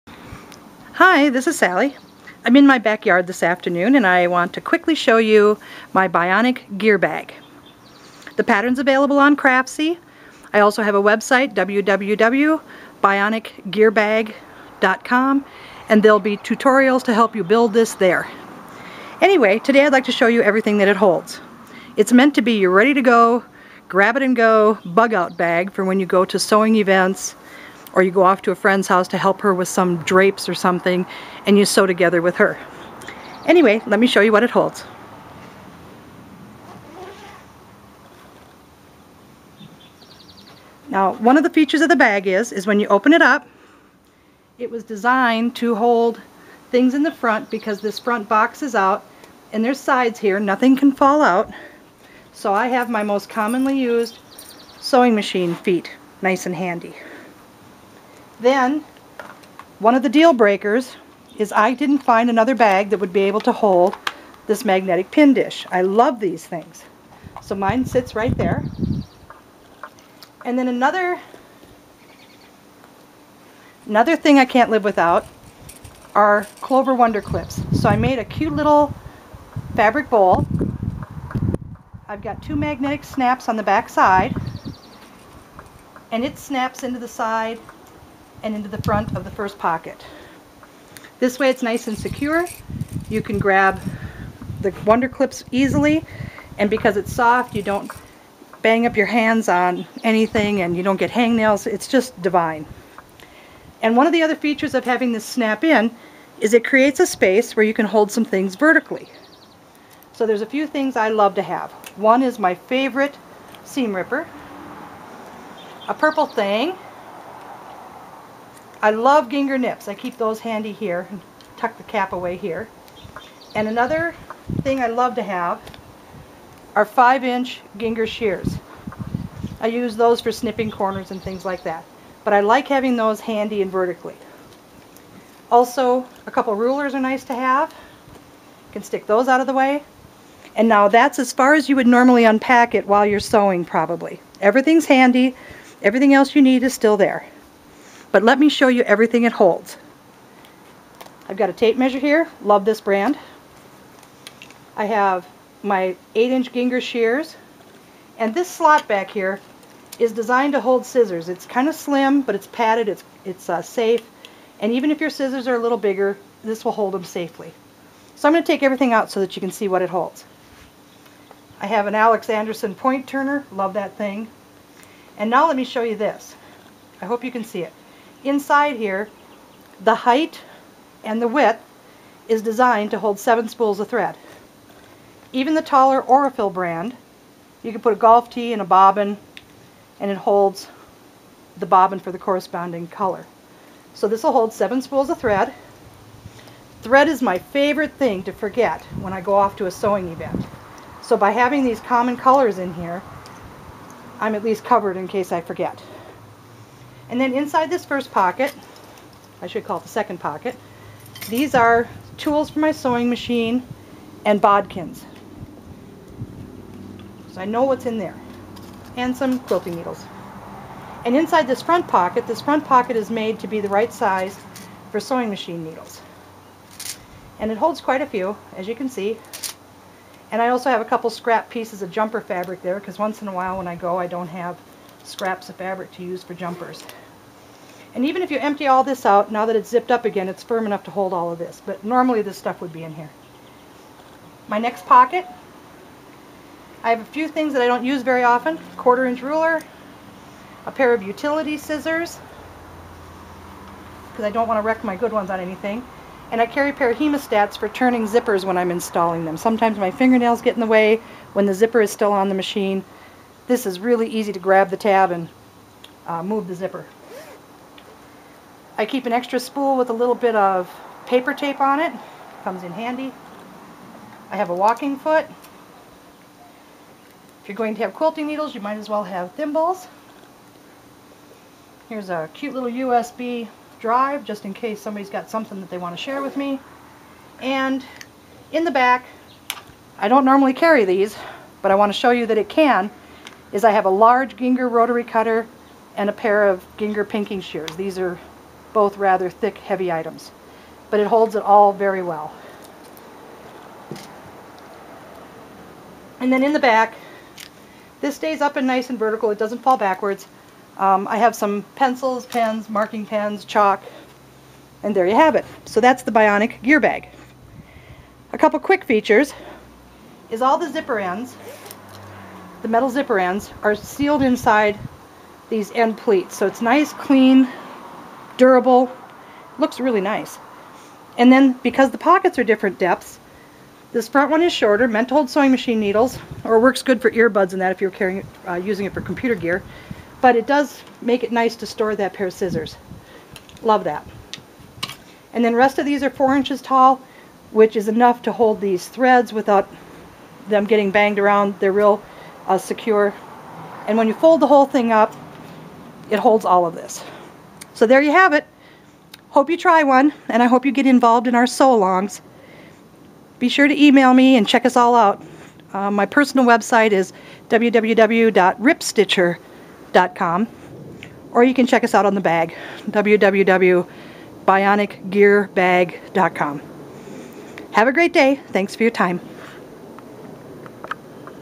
Bionic Gear Bag Demo
Bag designed to hold everything you need at workshops, sew-in's, retreats, or in your RV.   Also great for holding computer cords, etc.